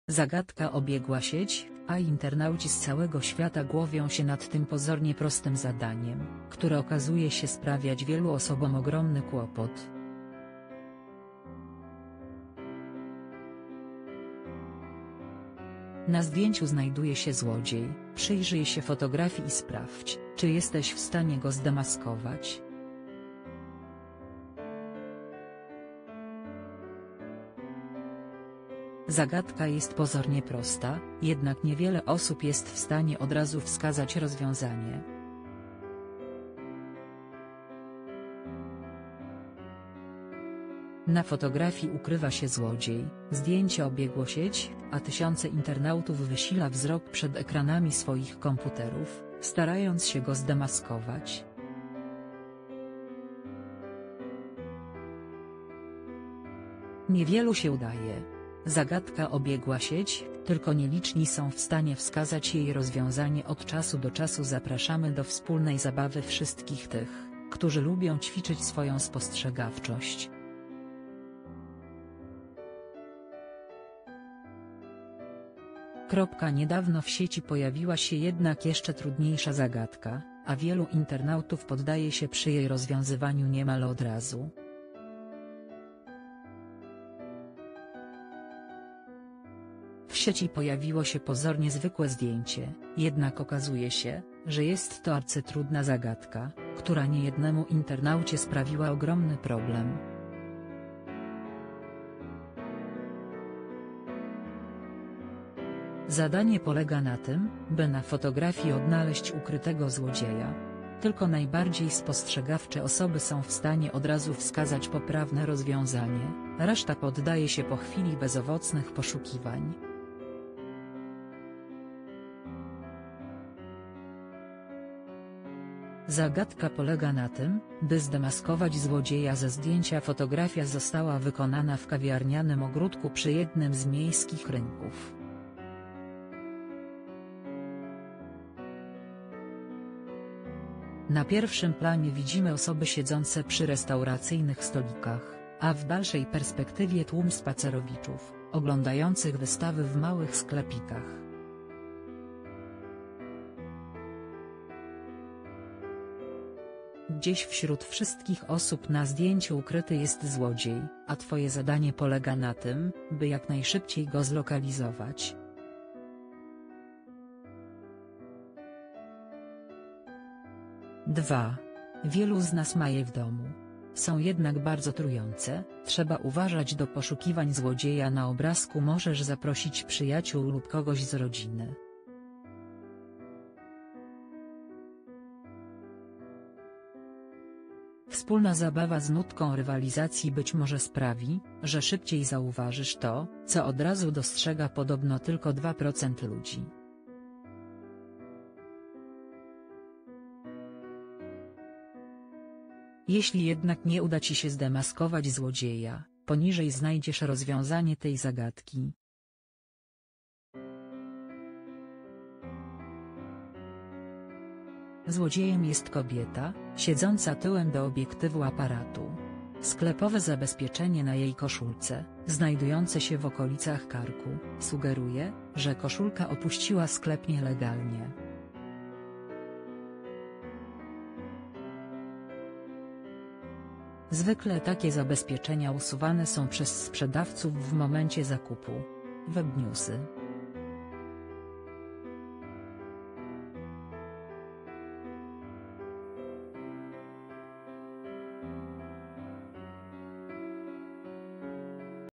Na zdjęciu ukrył się złodziej. Tylko 2% osób uda się go wskazać, reszta będzie męczyła się do upadłe
Zagadka obiegła sieć, a internauci z całego świata głowią się nad tym pozornie prostym zadaniem, które okazuje się sprawiać wielu osobom ogromny kłopot. Na zdjęciu znajduje się złodziej, przyjrzyj się fotografii i sprawdź, czy jesteś w stanie go zdemaskować.   Zagadka jest pozornie prosta, jednak niewiele osób jest w stanie od razu wskazać rozwiązanie. Na fotografii ukrywa się złodziej, zdjęcie obiegło sieć, a tysiące internautów wysila wzrok przed ekranami swoich komputerów, starając się go zdemaskować. Niewielu się udaje.  Zagadka obiegła sieć, tylko nieliczni są w stanie wskazać jej rozwiązanie   Od czasu do czasu zapraszamy do wspólnej zabawy wszystkich tych, którzy lubią ćwiczyć swoją spostrzegawczość. . Niedawno w sieci pojawiła się jednak jeszcze trudniejsza zagadka, a wielu internautów poddaje się przy jej rozwiązywaniu niemal od razu.   W sieci pojawiło się pozornie zwykłe zdjęcie, jednak okazuje się, że jest to arcytrudna zagadka, która niejednemu internaucie sprawiła ogromny problem. Zadanie polega na tym, by na fotografii odnaleźć ukrytego złodzieja. Tylko najbardziej spostrzegawcze osoby są w stanie od razu wskazać poprawne rozwiązanie, reszta poddaje się po chwili bezowocnych poszukiwań.  Zagadka polega na tym, by zdemaskować złodzieja ze zdjęcia      Fotografia została wykonana w kawiarnianym ogródku przy jednym z miejskich rynków. Na pierwszym planie widzimy osoby siedzące przy restauracyjnych stolikach, a w dalszej perspektywie tłum spacerowiczów, oglądających wystawy w małych sklepikach. Gdzieś wśród wszystkich osób na zdjęciu ukryty jest złodziej, a twoje zadanie polega na tym, by jak najszybciej go zlokalizować.       2. Wielu z nas ma je w domu. Są jednak bardzo trujące, trzeba uważać   Do poszukiwań złodzieja na obrazku możesz zaprosić przyjaciół lub kogoś z rodziny. Wspólna zabawa z nutką rywalizacji być może sprawi, że szybciej zauważysz to, co od razu dostrzega podobno tylko 2 procent ludzi. Jeśli jednak nie uda ci się zdemaskować złodzieja, poniżej znajdziesz rozwiązanie tej zagadki.    Złodziejem jest kobieta, siedząca tyłem do obiektywu aparatu. Sklepowe zabezpieczenie na jej koszulce, znajdujące się w okolicach karku, sugeruje, że koszulka opuściła sklep nielegalnie. Zwykle takie zabezpieczenia usuwane są przez sprzedawców w momencie zakupu.     Webniusy